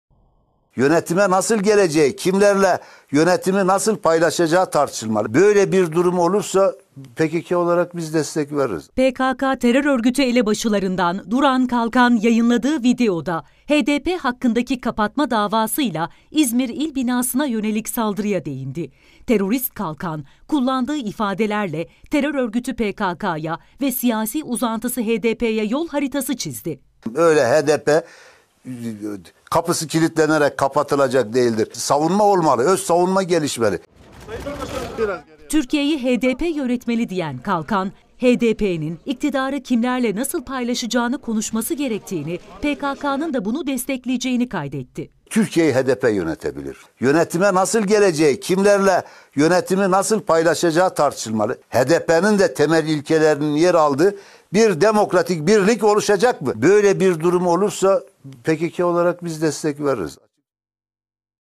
PKK elebaşı Duran Kalkan HDP'ye yol haritası çizdi
Terör örgütü PKK elebaşlarından Duran Kalkan, kapatma davası açılan HDP’ye yol haritası çizdi. “Türkiye’yi HDP yönetmeli” diyen Kalkan, HDP’nin iktidarı kimlerle nasıl paylaşacağını konuşması gerektiğini, PKK’nın da HDP’yi destekleyeceğini söyledi.